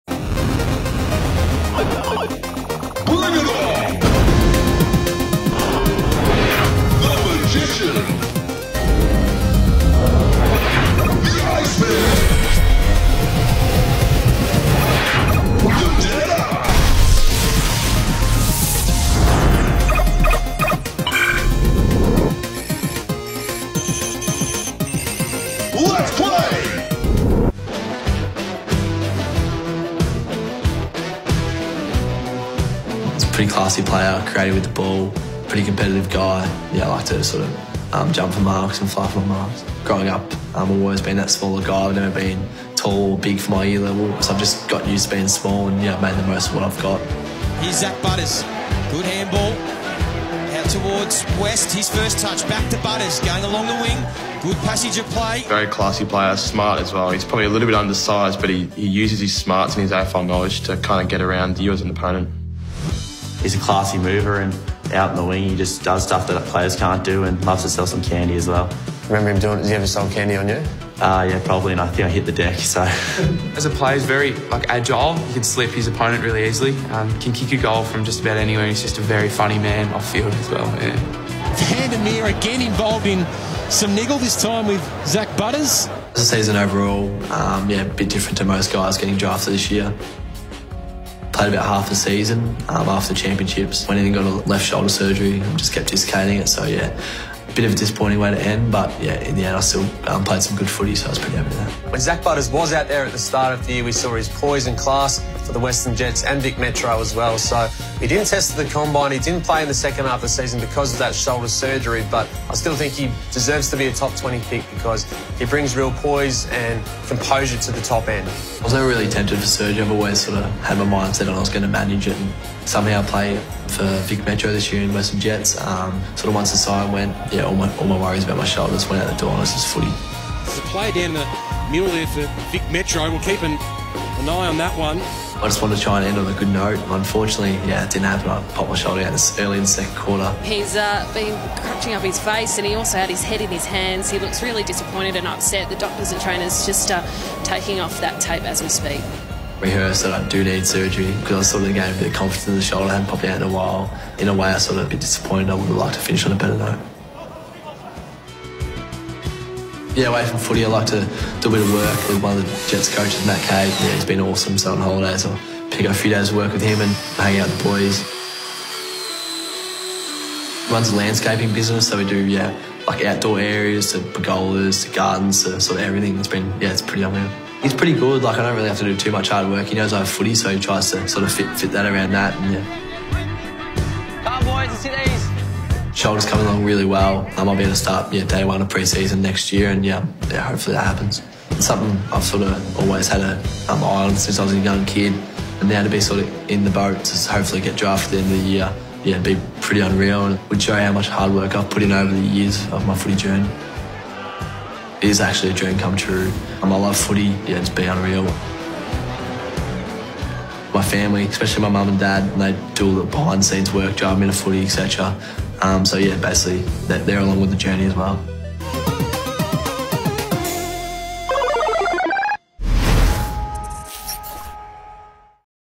'The cool head': Get to know Zak Butters | Tomorrow's Heroes | 2018 | AFL
Plans were already in place for Zak Butters to have season-ending shoulder surgery after Vic Metro's final game of the NAB AFL Under-18 Championships.

The midfielder from the Western Jets had been carrying the complaint for some time but had hoped to impress at national level, get through the carnival and then go under the knife. It nearly went to plan, until he suffered a setback with the shoulder early in the title-deciding game against South Australia and had his final outing cut short.